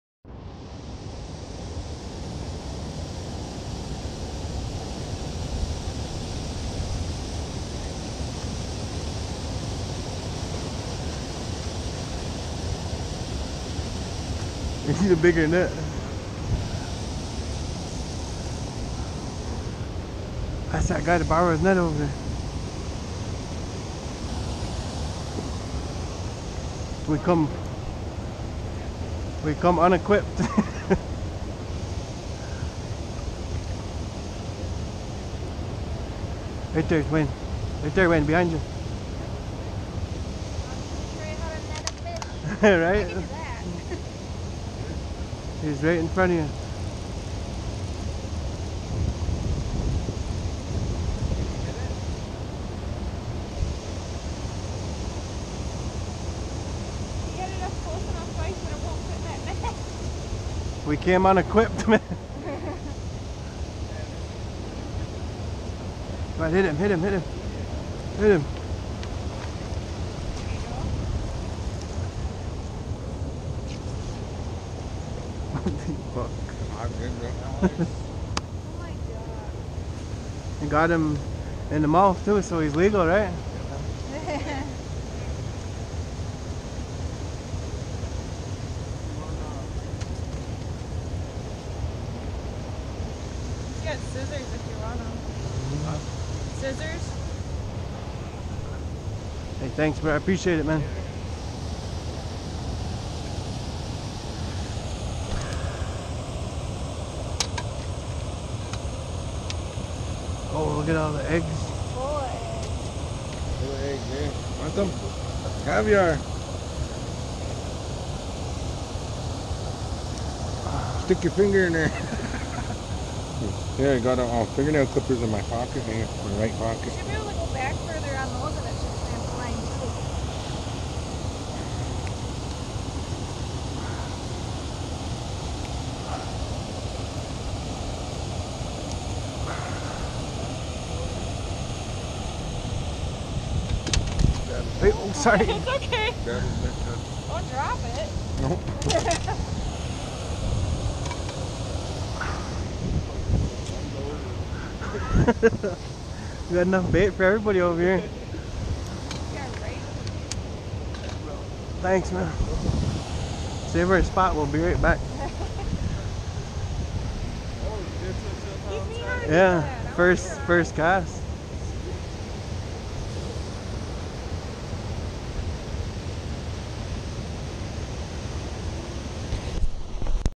Mishicot Dam Salmon run!
Slayed my first salmon off shore at the little honey hole in mishicot, WI. Created with the GoPro App